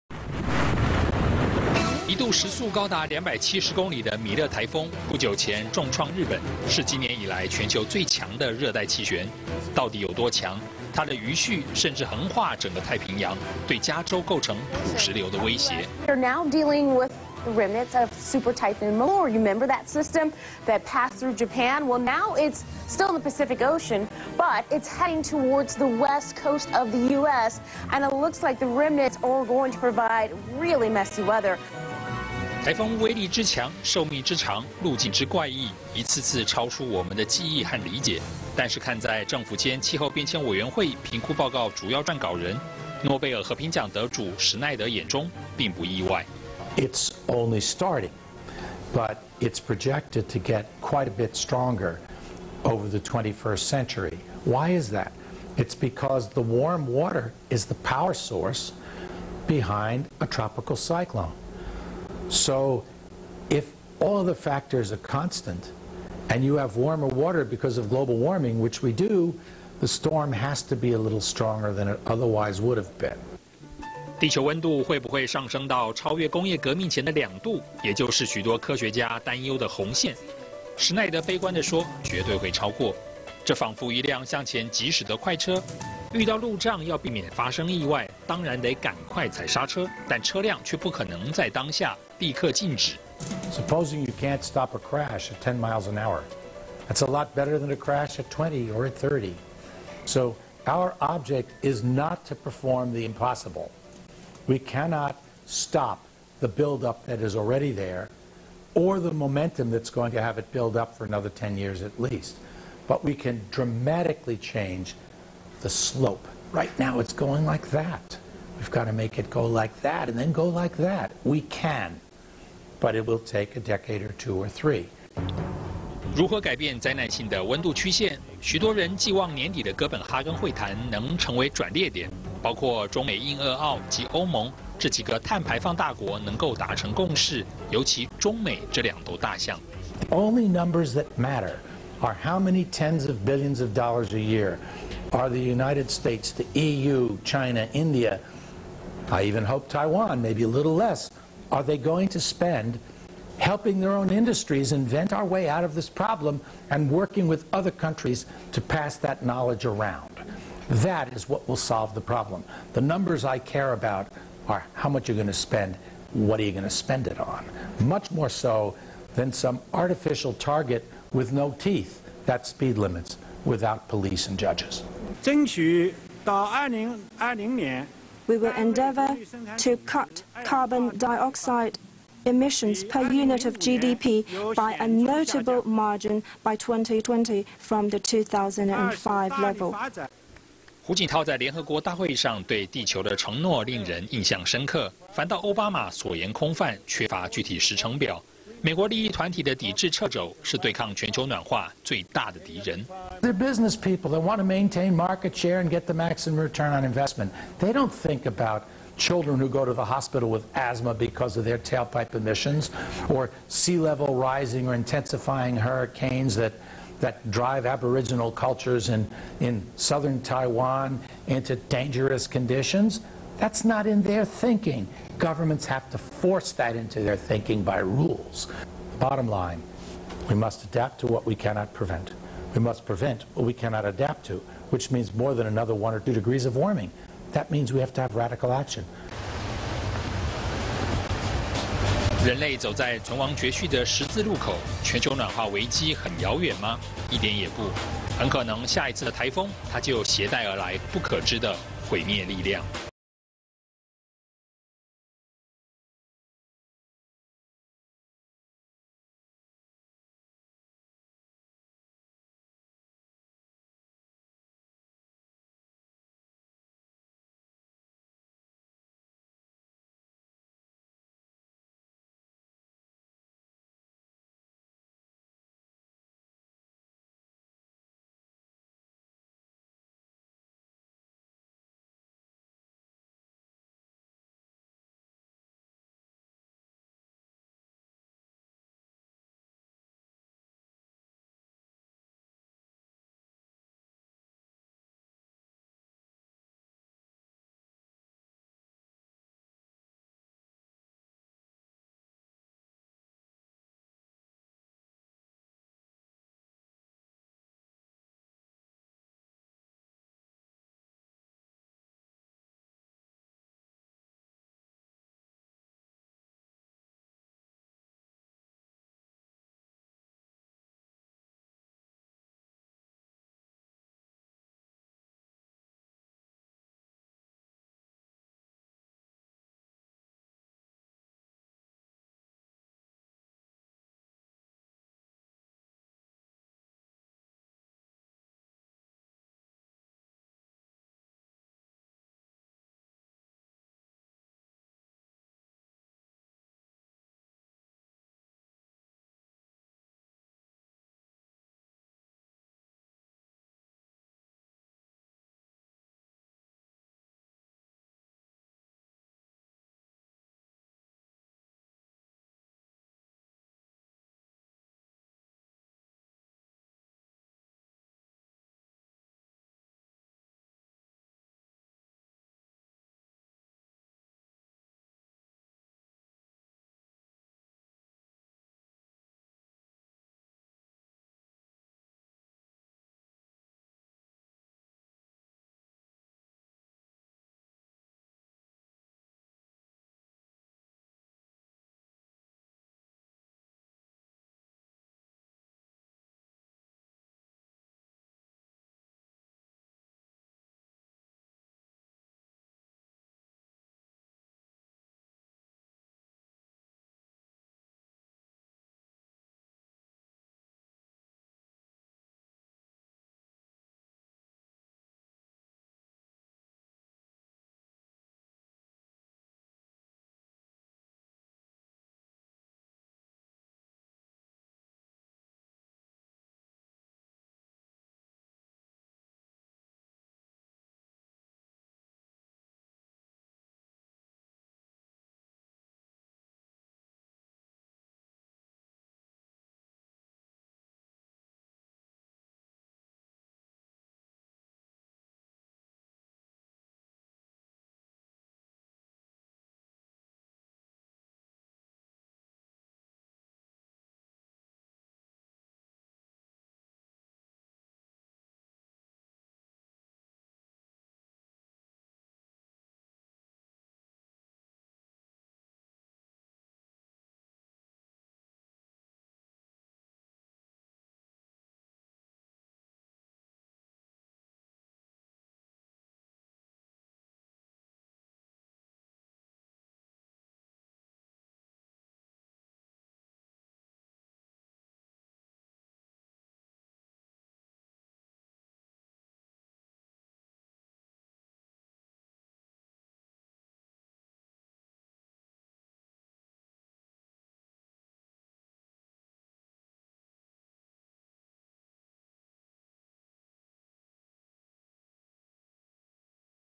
創世科技-全球暖化：史奈德教授的說明(霹靂創世錄pilicreateworld.tw-blog.com)
影片來源:中天新聞